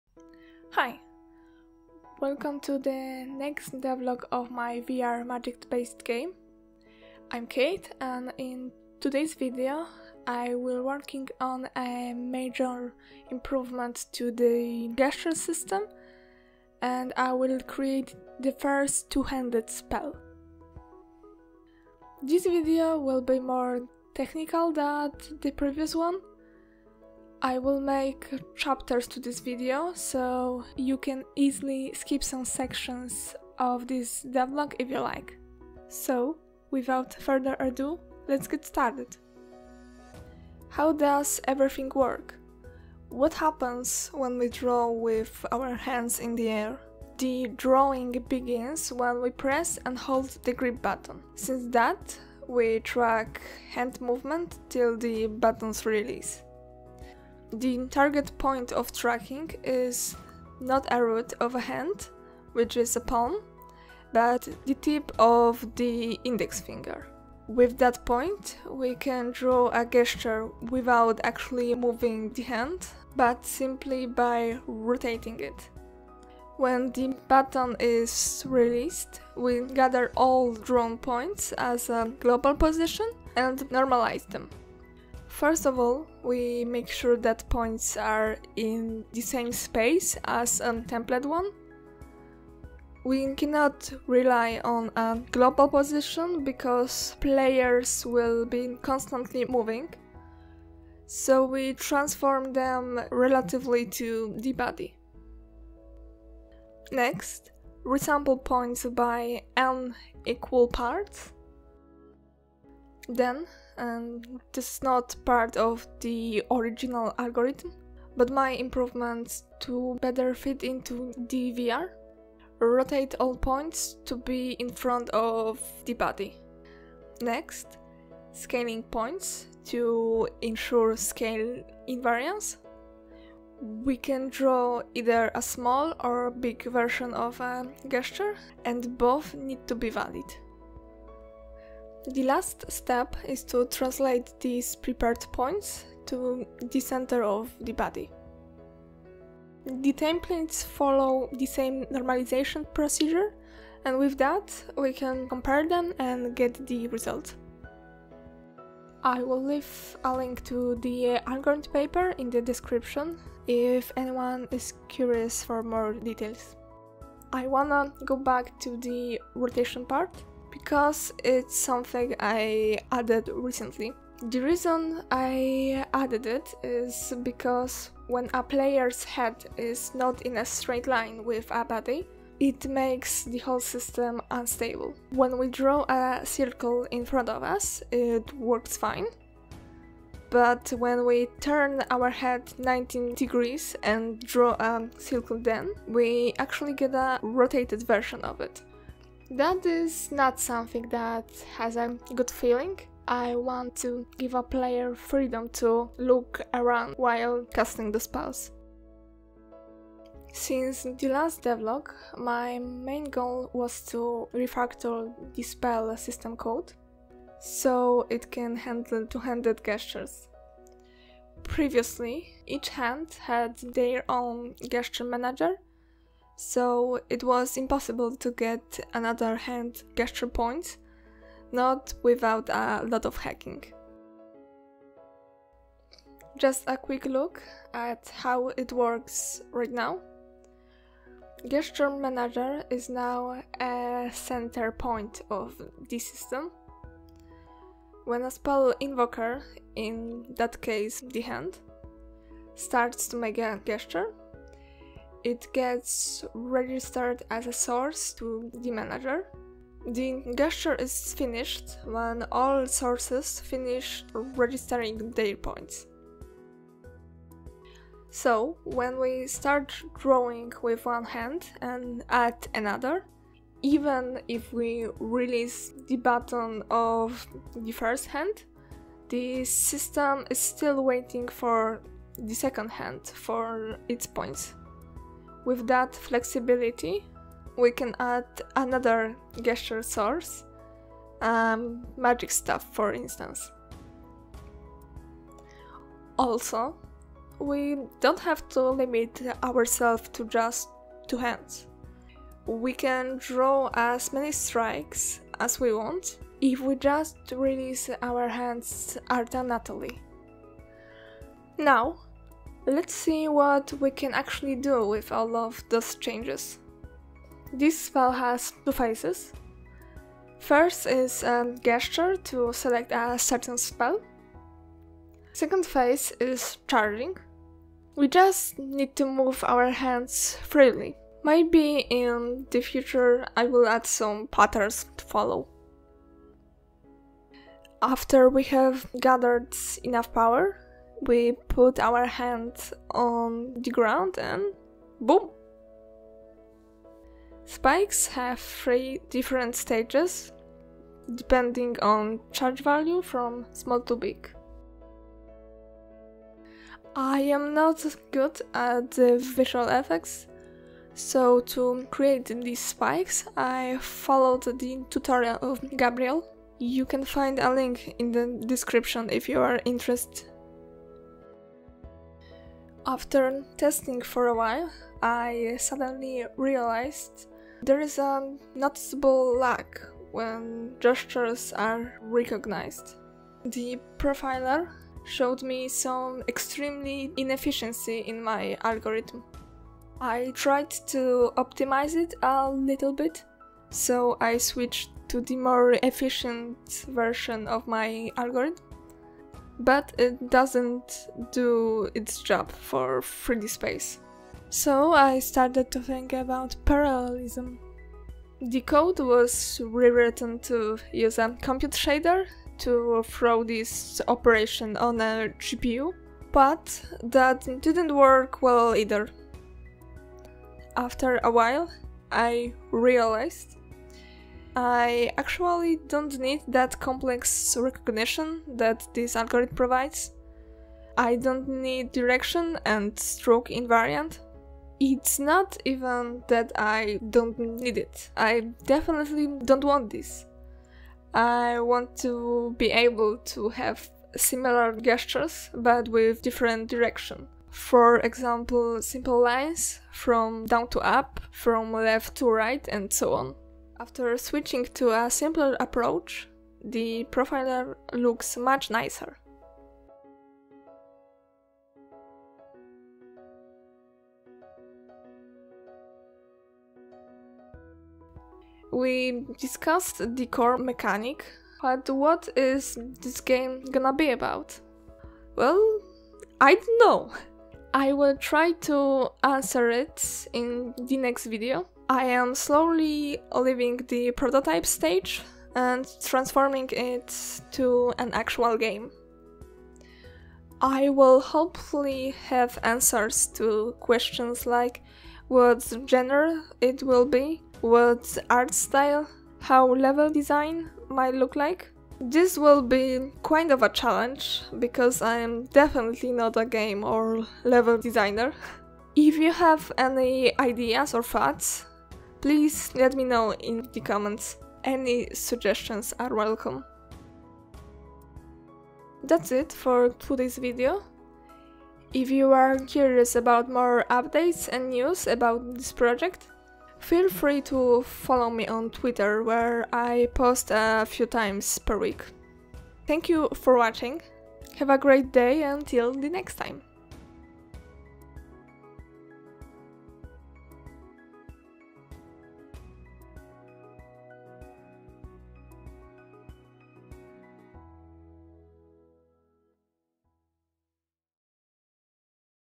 How gesture recognition works in my VR game | Devlog #2
Hi! Welcome to the second devlog. I got myself a new microphone so quality should be better.
In this episode, we will talking about how gesture recognition works and what optimization and refactor was made for handle two-handed spells.

Chapters:
0:00 Introduction
0:40 How is works?
1:24 Normalization process
3:30 Refactor
4:26 Multistrokes
5:12 Showcase of two-handed spell
6:10 Performance problem
7:50 What's next?